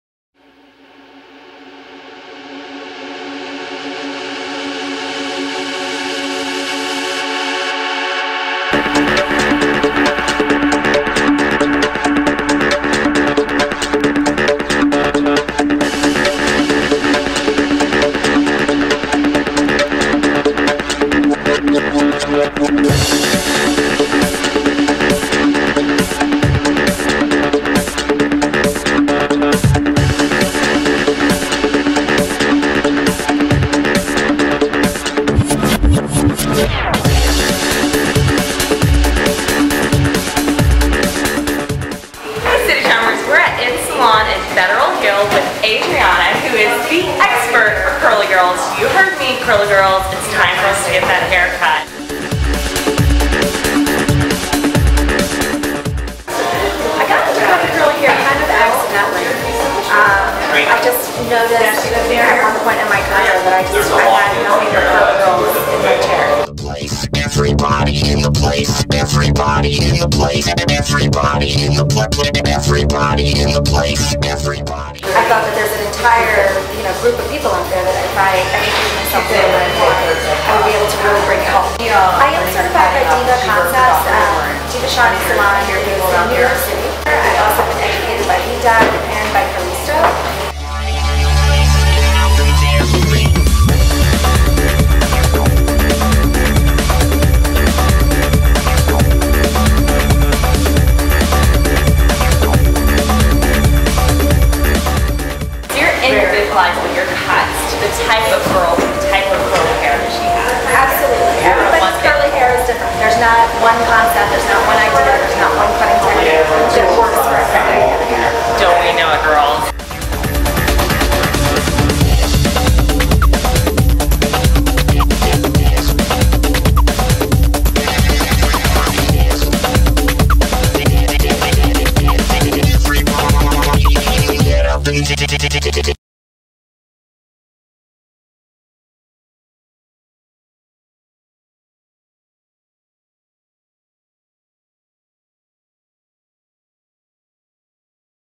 City Charmer | CBS | M Salon - Federal Hill, Baltimore | Curly Girl Hair Cut
Curly girls are thrilled to see M Salon style their way into the Charming Business Spotlight! Adriana, our Curly Expert who is Deva Certified, will teach you how to rehab, treat, and flaunt your beautiful curls with less effort and more curl. Frizzy days are gone and beautiful locks of curls remain.